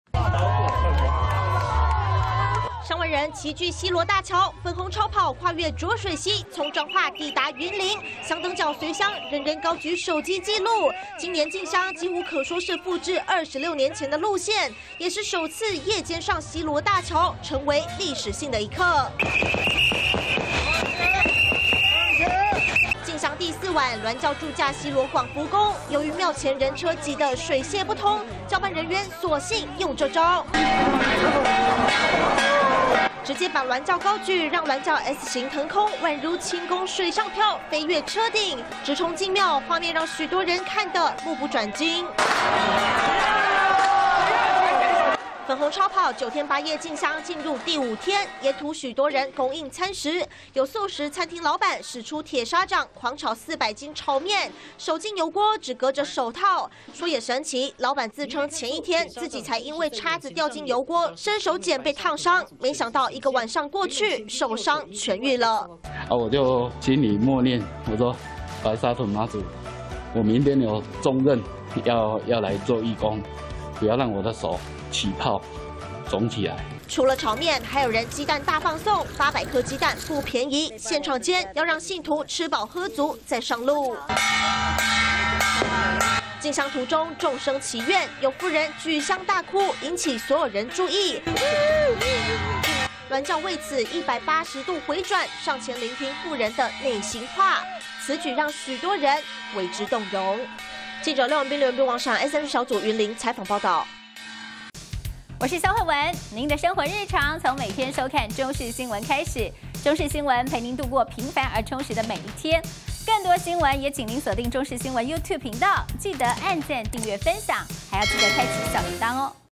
首次夜奔西螺大橋 粉紅超跑"S型騰空飛躍"越車陣│中視新聞 20230216
白沙屯媽祖進香上萬人齊聚西螺大橋前，成為歷史性的一刻，因為這是白沙屯媽祖，首次夜間上西螺大橋，而當晚駐駕西螺廣福宮，要進廟前，因為人車太多，轎班還高舉鑾轎，讓鑾轎飛躍車頂，宛如輕功水上漂，另外，進香途中許多人供應餐食，有素食餐廳老闆，使出鐵砂掌，手伸進油鍋炒麵，畫面超震撼。